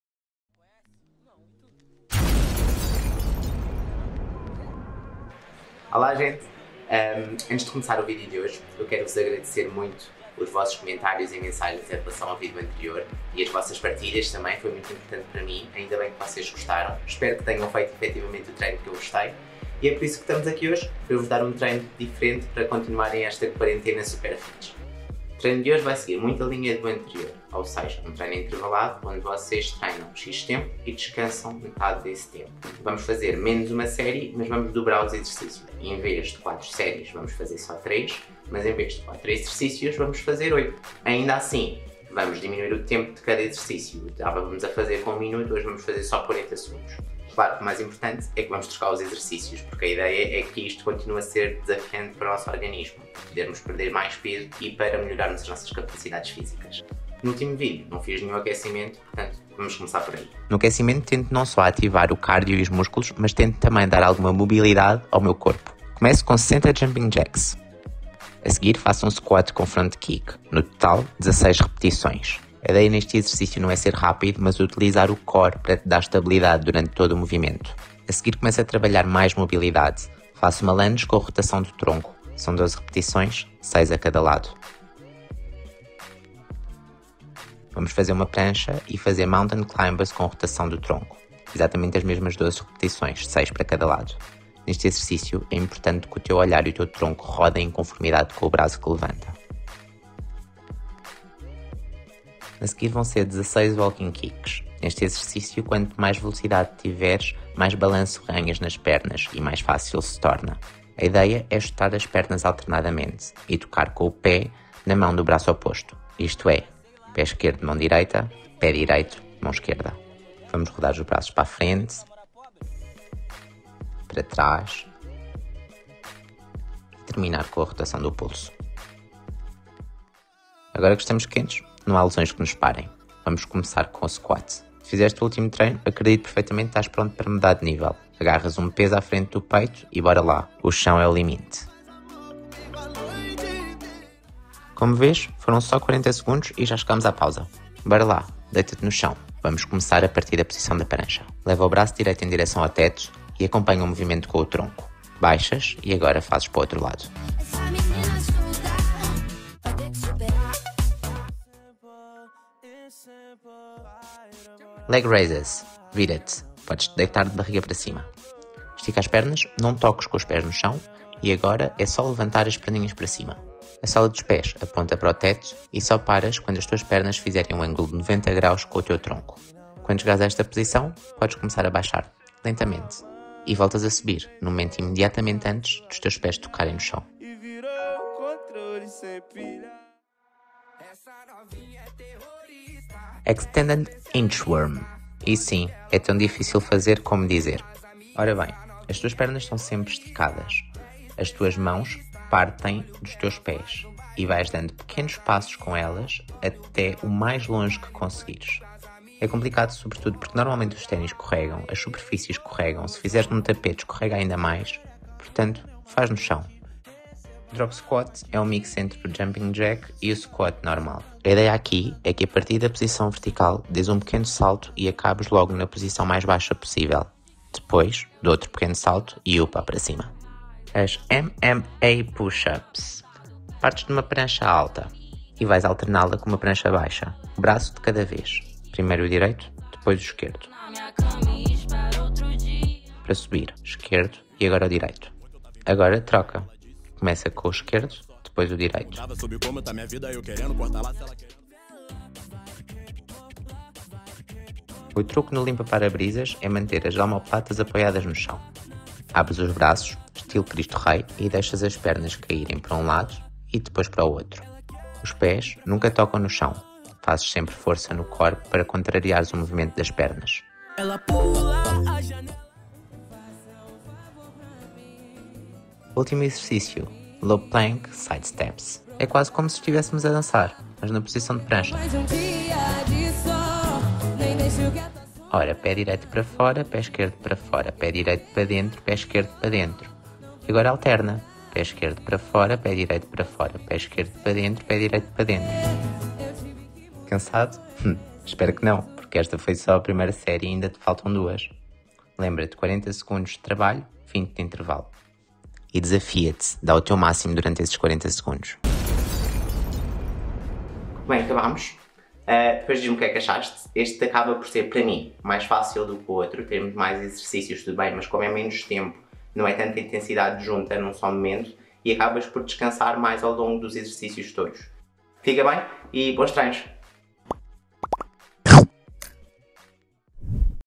Treino para Quarentena II • Sem material para fazer em casa
Depois do último video de TreinoDeQuarentena ter recebido bom feedback achei que estava na altura de trocar o treino 💪 e de vos dar alguns exercícios novos para não perderem o ritmo!
Mantendo a linha do anterior, este é mais uma vez um treino intervalado ⏱com exercícios que podes fazer facilmente em qualquer lado sem necessitares de muito espaço.
Desta vez proponho-te 3 circuitos de 8 exercícios. Trabalhas 40 segundos por exercício e descansas 20 entre cada.
A ideia é concluir 3 vezes o seguinte plano:
40’’ SQUAT
20’’ PAUSA
40’’ T LOW PLANK
20’’ PAUSA
40’’ LEG RAISES
20’’ PAUSA
40’’ EXTENDED INCHWORM
20’’ PAUSA
40’’ DROP SQUAT
20’’ PAUSA
40’’ MMA PUSH UPS
20’’ PAUSA
40’’ LIMPA PÁRA BRISAS
20’’ PAUSA
40’’ LOW PLANK SIDE STEPS
20’’ PAUSA

Espero que tenhas gostado! Fico à espera do teu feedback e de ver esses stories de QuarentenaFitness 🏋️‍♂️
WorkIt e bons treinos! 💪